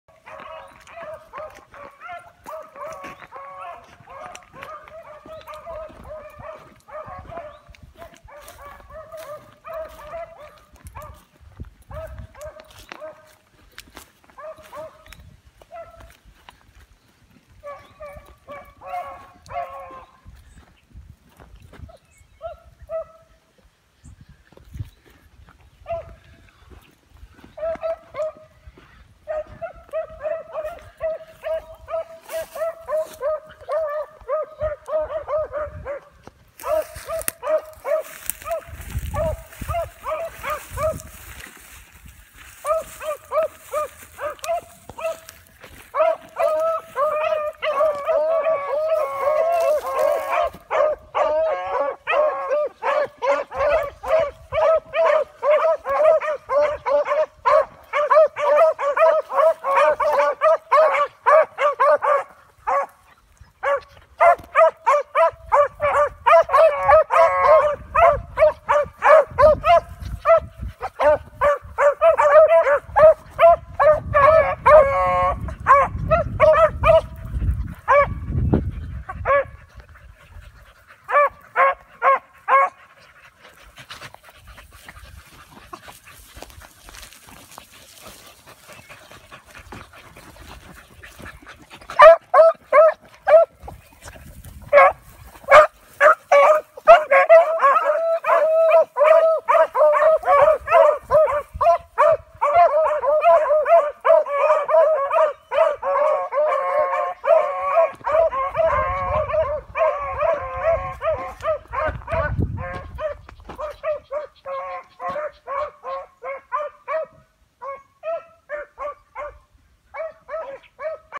UBGF beagles 5/29/18
Shorts Happy Jack male, Goodwater Guinea granddaughter, E-Z-Go female and Thorn Gap Dark Bad Boy Spike female.